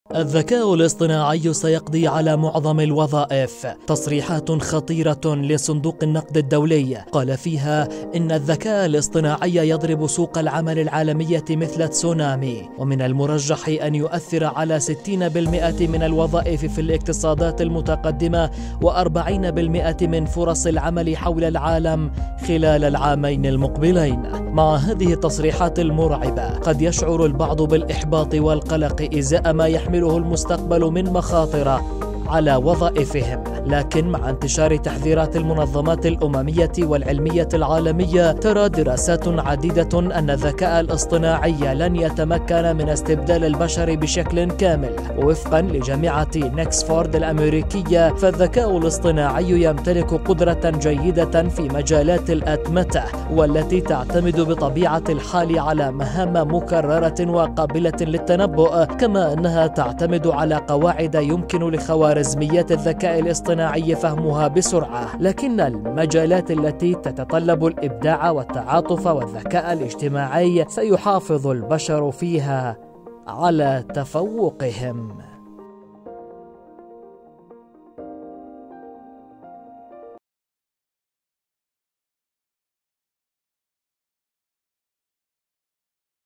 الذكاء الاصطناعي يسرق وظائف البشر!
تصريحات خطيرة لصندوق النقد الدولي، قال فيها إن الذكاء الاصطناعي يضرب سوق العمل العالمية مثل تسونامي ومن المرجح أن يؤثر على ستين بالمئة من الوظائف في الاقتصادات المتقدمة وأربعين بالمئة من فرص العمل حول العالم خلال العامين المقبلين.